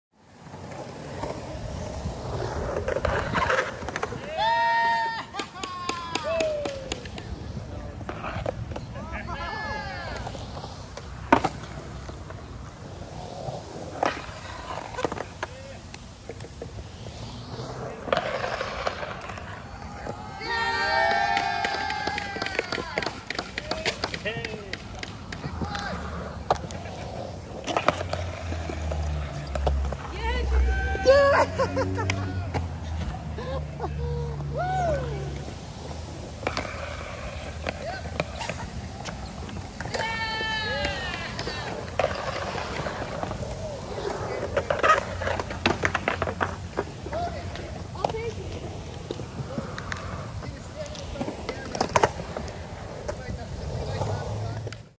Antigonish Skatepark Aug. 25, 2021 - OG Skate Night
The Antigonish Skatepark opened in November 2019. It is a free and public park and welcomes anyone who wants to skateboard, bike or scooter for the kids who haven't converted to skateboards yet. The Antigonish Skatepark is located at 42 West Street in Antigonish, Nova Scotia. It is front and center in the town because we wanted to showcase the action, activity and talent in Antigonish.

Thank you to The Town of Antigonish, Municipality of the County of Antigonish, St. FX University, The Antigonish Skatepark Association